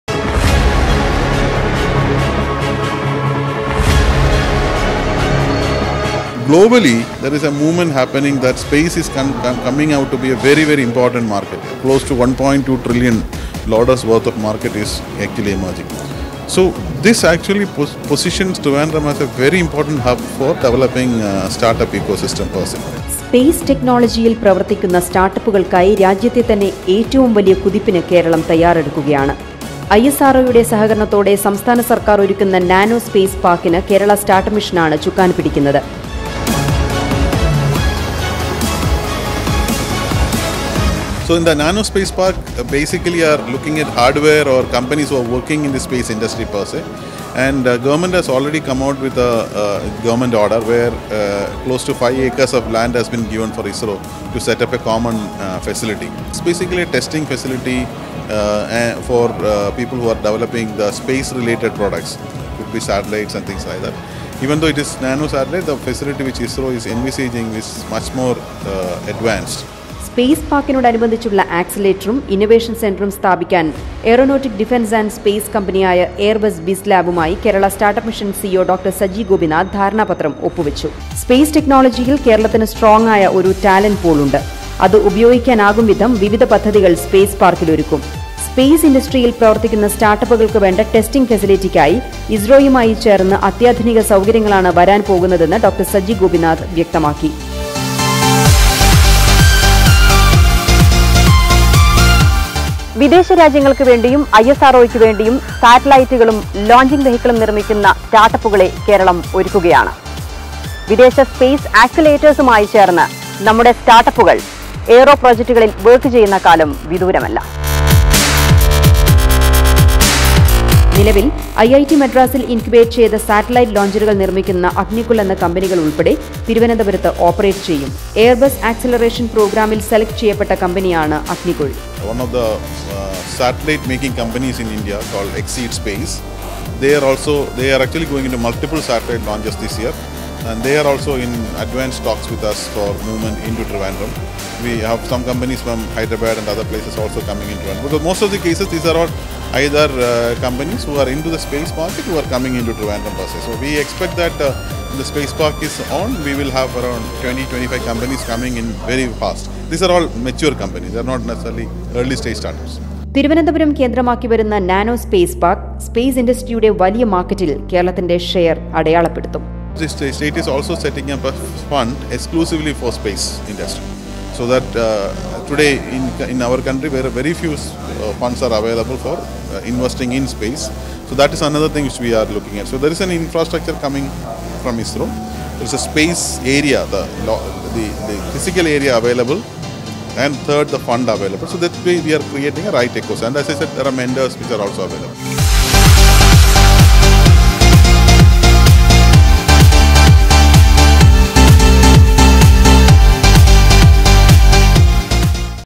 Kerala Government & ISRO join together for Spacetech park to promote space technology startups
സ്പെയ്സ് ടെക്നോളജി സ്റ്റാര്‍ട്ടപ്പുകള്‍ക്കായി രാജ്യത്തെ തന്നെ ഏറ്റവും വലിയ കുതിപ്പിന് കേരളം തയ്യാറെടുക്കുകയാണ്. സ്‌പേസ് ഇന്‍ഡസ്ട്രിയില്‍ പ്രവര്‍ത്തിക്കുന്ന സ്റ്റാര്‍ട്ടപ്പുകള്‍ക്ക് വേണ്ട ടെസ്റ്റിംഗ് ഫെസിലിറ്റിക്കായി കടഞഛ യുമായി ചേര്‍ന്ന് അത്യാധുനിക സൗകര്യങ്ങളാണ് വരാന്‍ പോകുന്നതെന്ന് കേരള സ്റ്റാര്‍ട്ടപ്പ് മിഷന്‍ സിഇഒ ഡോ. സജി ഗോപിനാഥ് വ്യക്തമാക്കി. ISRO യുടെ സഹകരണത്തോടെ സംസ്ഥാന സര്‍ക്കാര്‍ ഒരുക്കുന്ന നാനോ സ്‌പേസ് പാര്‍ക്കിന് കേരള സ്റ്റാര്‍ട്ടപ്പ് മിഷനാണ് ചുക്കാന്‍ പിടിക്കുന്നത്.  സ്‌പേസ് ടെക്‌നോളജിയില്‍ കേരളത്തിന്റെ സ്‌ട്രോങ് ടാലന്റ് പൂള്‍ ഉപയോഗിക്കാനാകും വിധം വിവിധ പദ്ധതികള്‍ സ്‌പേസ് പാര്‍ക്കില്‍ ഒരുക്കും.